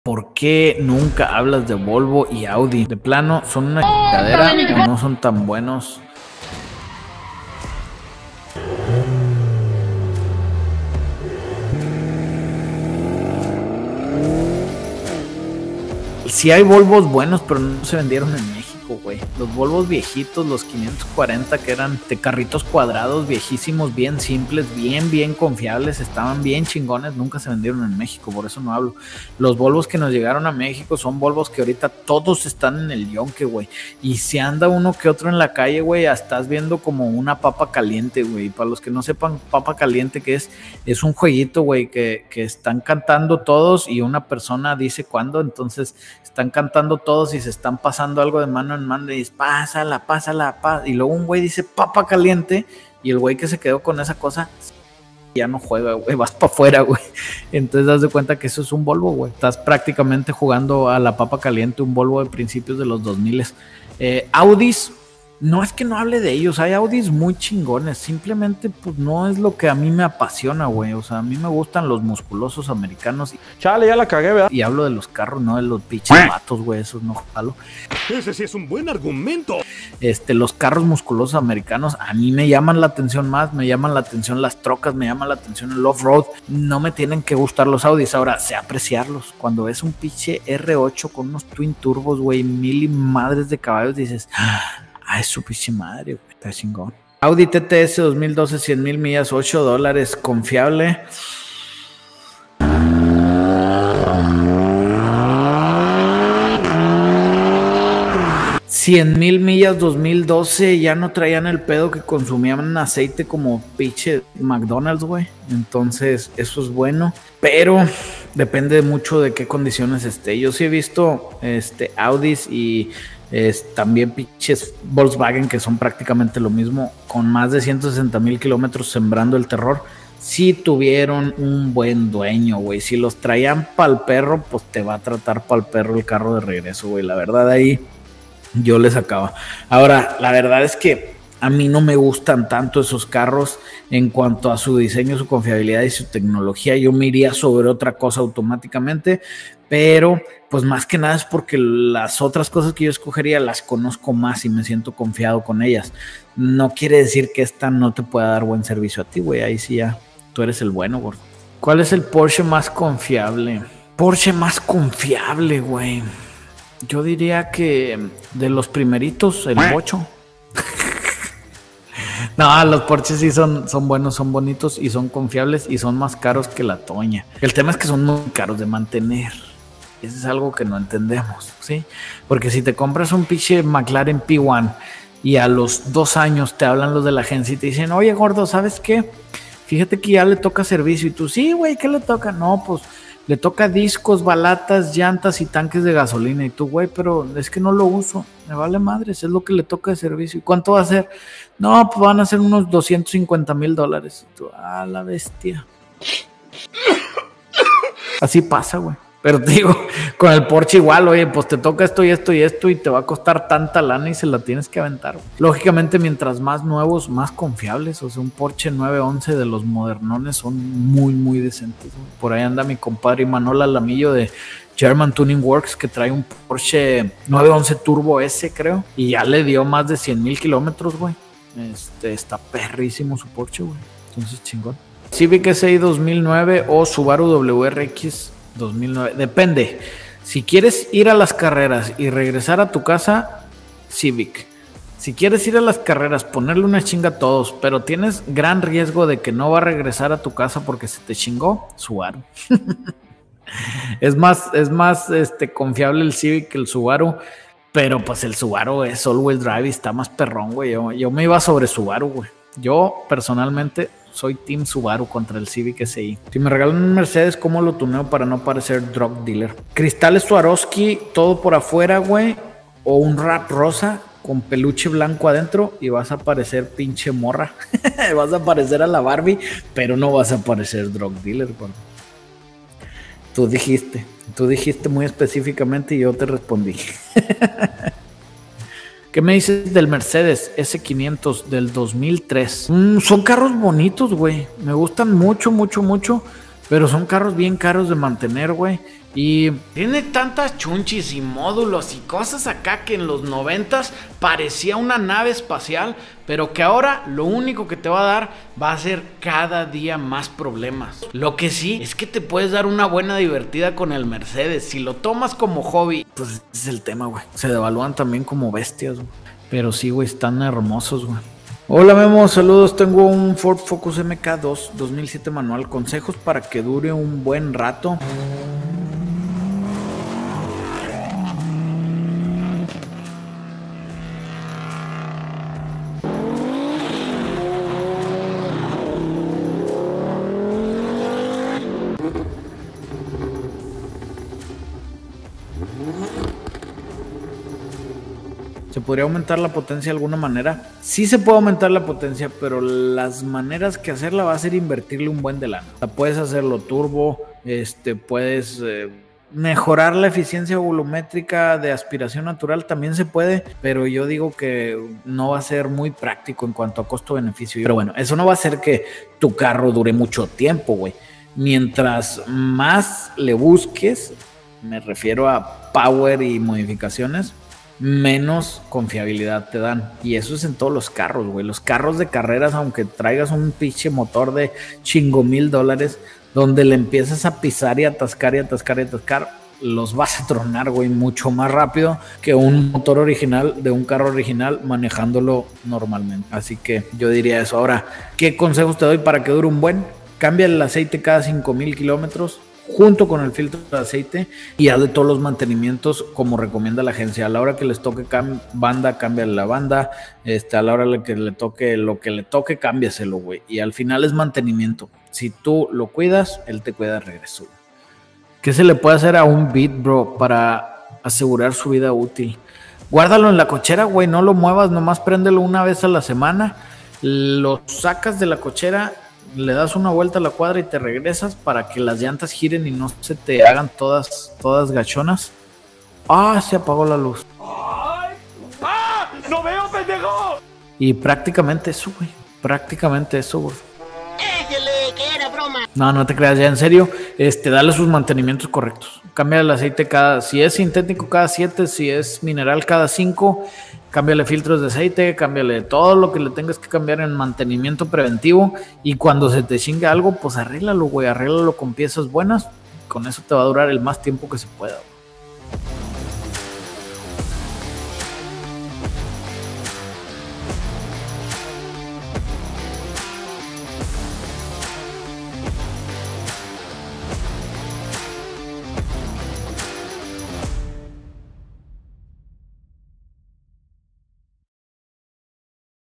Por Que Nunca Hablo de Volvo y Audi??' /// Esto Es Lo Que Opino Ellas
En el atiguo Livestream me preguntaron del por que casi nunca hablo de Volvo y Audi; Aquí les respondo si me gustan estas marcas, y ademas incluyo mi opinon sobre Porsche y cual es el más confiable...

Necesitas cotizar un Swap o una Restauración Profesional, llena el formulario de este link:

Colaboraciones